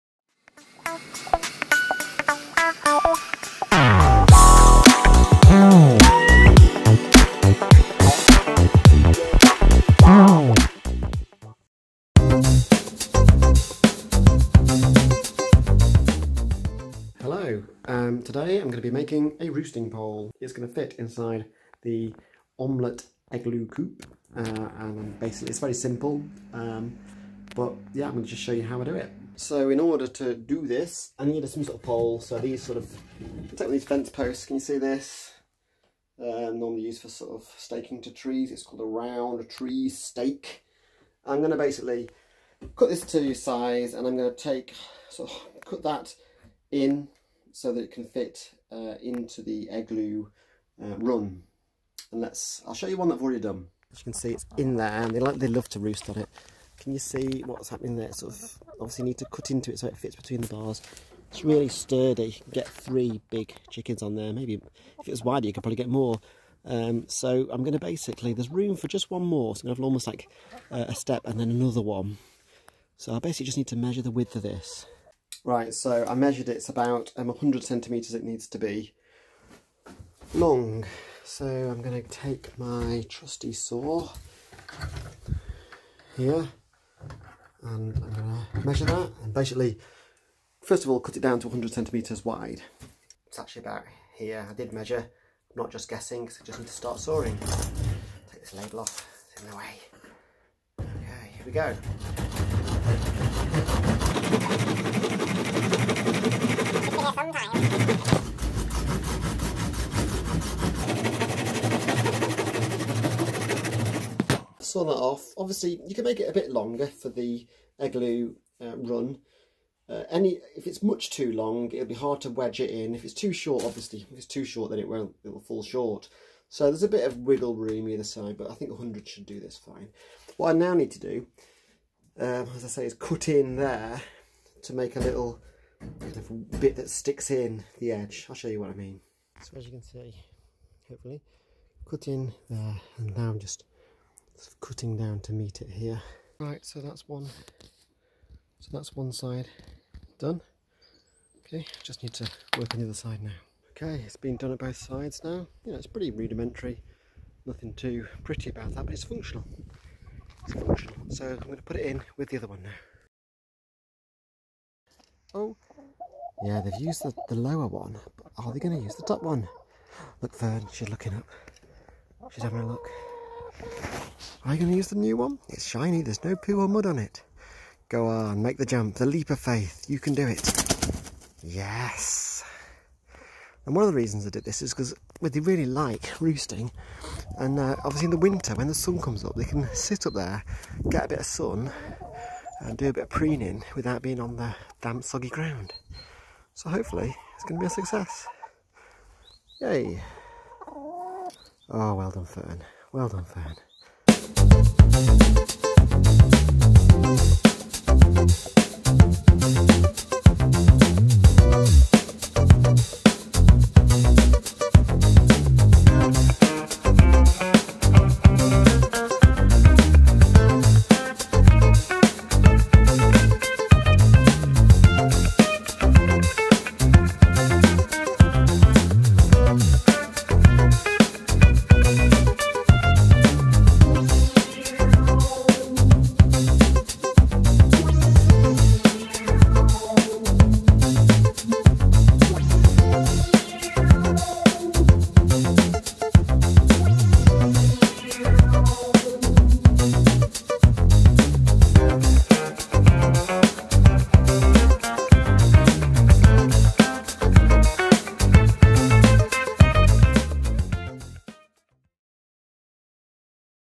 How to make a roosting bar for your chickens in an omlet eglu coop run
I show you how to create a simple and effective roosting bar to put inside your omlet eglu run. It's quick and cheap to do and your chickens will thank you for it!